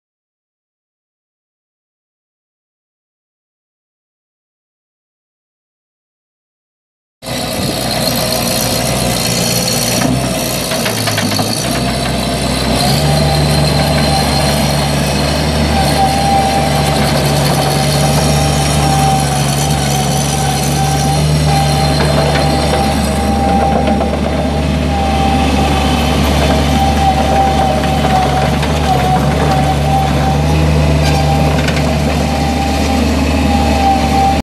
2008 Bobcat MT55 compact utility loader for sale | no-reserve Internet auction March 30, 2017
2008 Bobcat MT55 compact utility loader
800 hours on meter
Kubota 0.898L three cylinder diesel engine
18.5 kW
Hydrostatic transmission
Auxiliary hydraulics
43.5"W bucket
10"W tracks
Ride-on platform included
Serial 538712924
It's located in Lecompton, KS, and we're selling it to the highest bidder regardless of price.